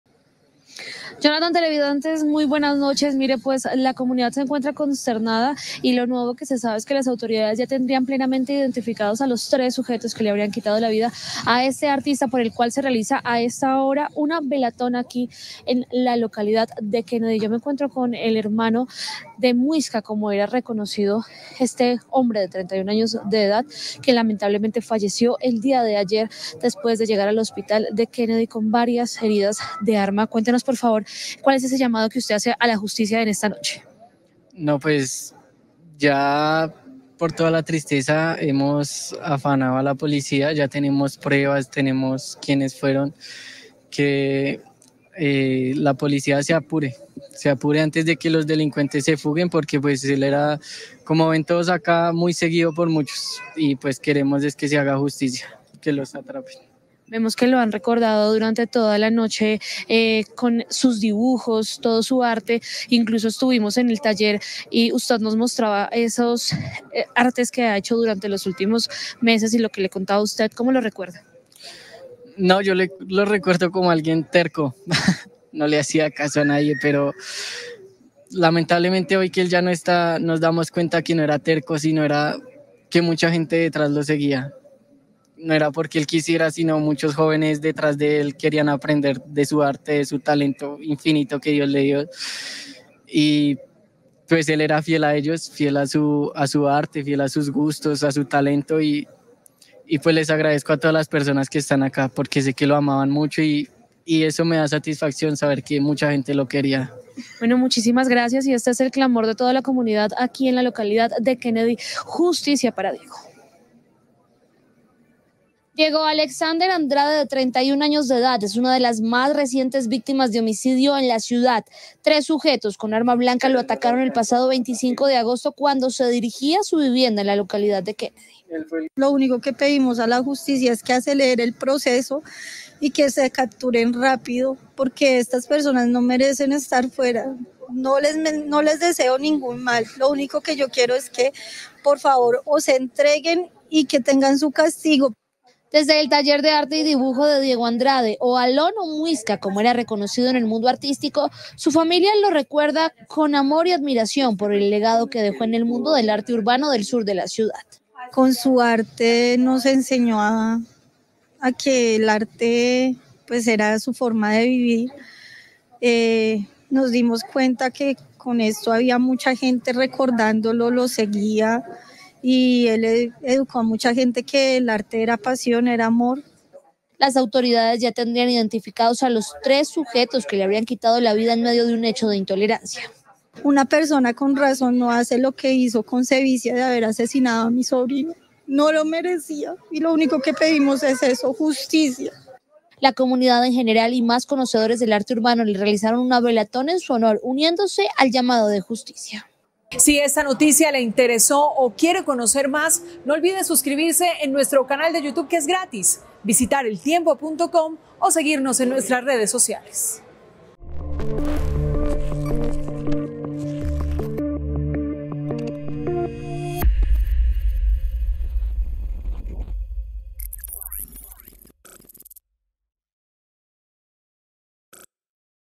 Familia de Diego Andrade, artista asesinado en Kennedy, pide justicia | El Tiempo
Él era Diego Alexander Andrade, un reconocido artista de la localidad de Kennedy que fue asesinado hace 36 horas. Se trata de una de las más recientes víctimas de homicidio en Bogotá. Las autoridades ya identificaron a tres agresores.